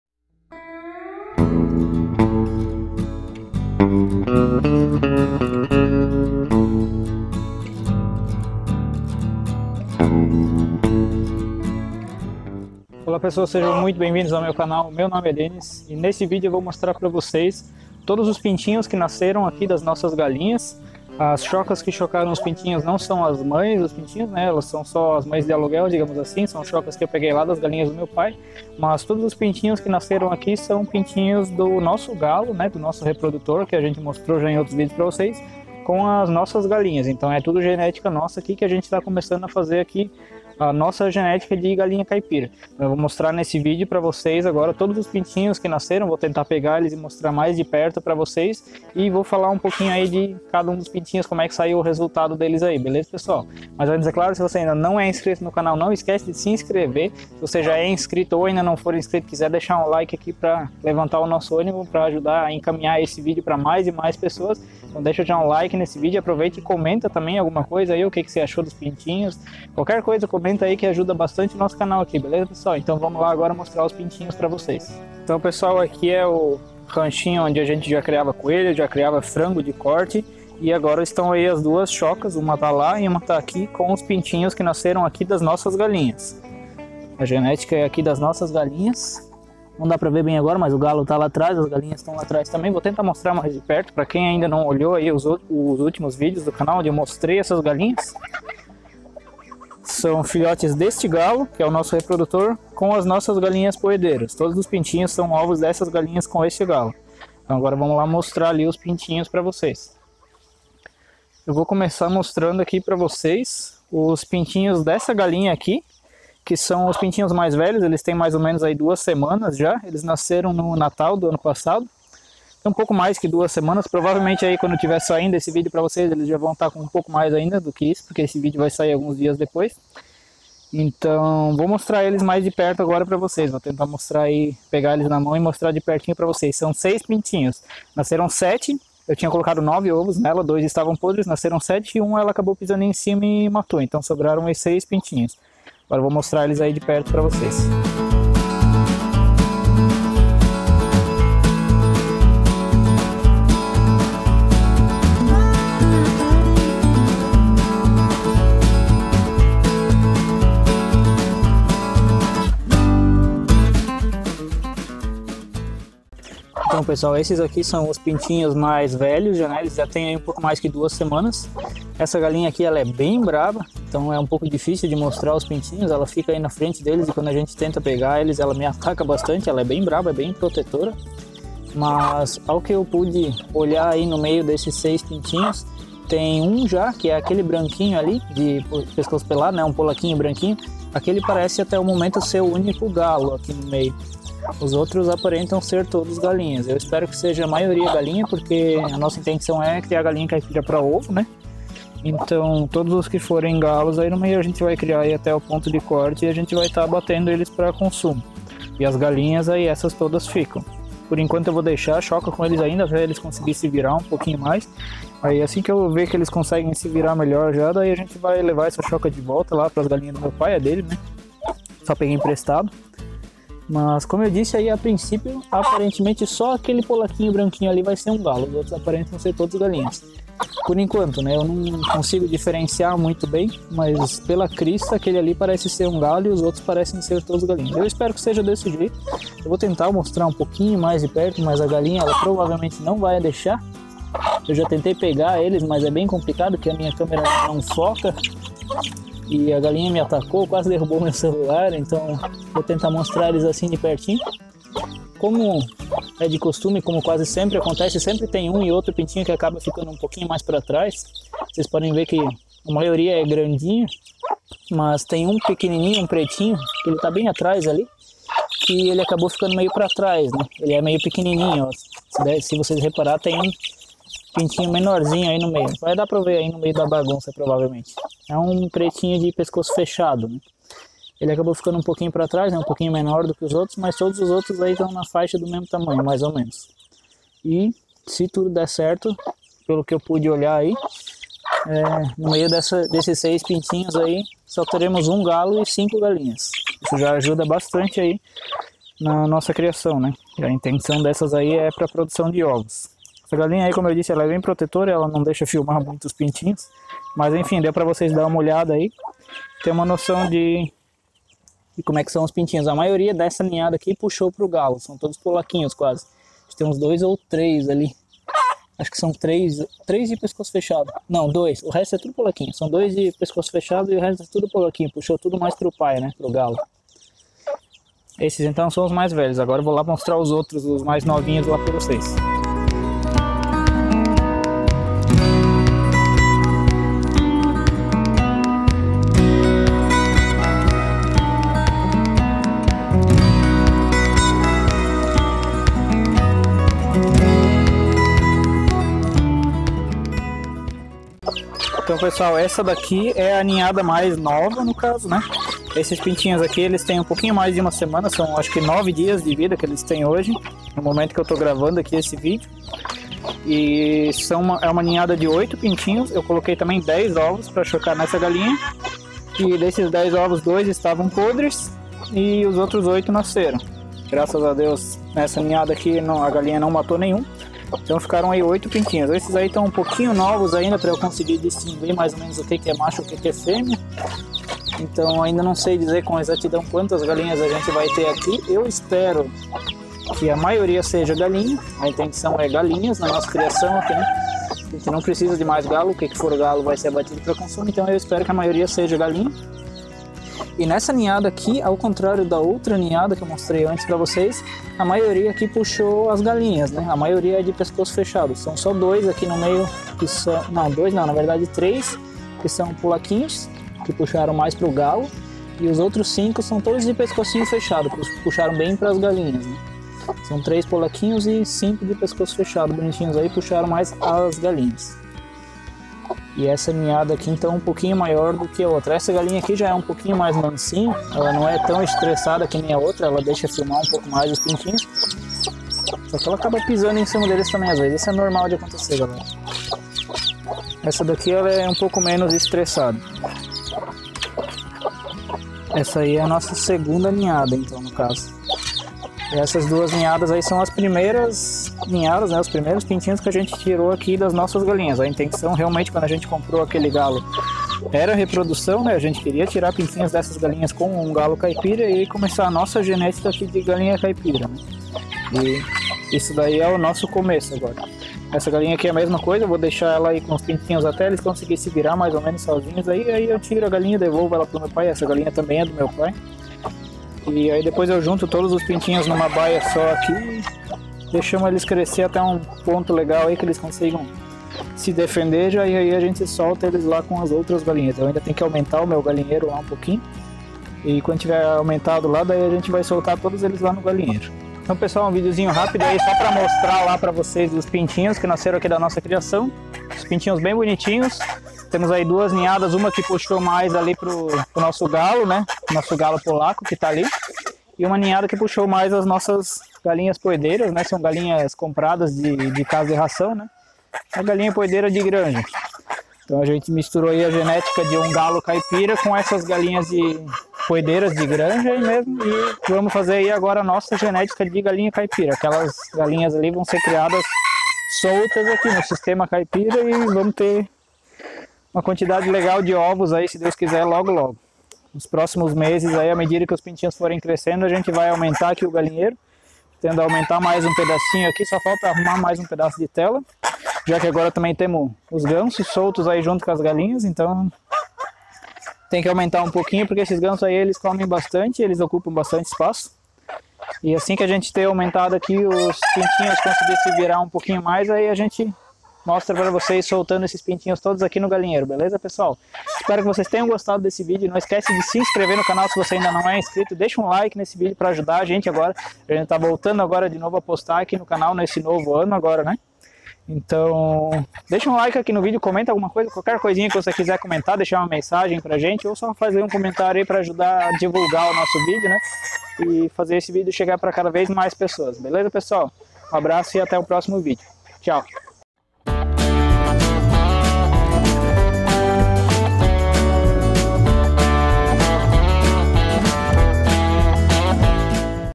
MOSTRANDO OS NOVOS PINTINHOS CAIPIRAS QUE NASCERAM NO SÍTIO / Mini Sítio
Neste vídeo mostro as duas ninhadas de pintinhos caipiras que nasceram no nosso mini sítio, esses pintinhos são o início da nossa genética caipira, são cruzamento das nossas galinhas poedeiras com o nosso galo caipira polaco.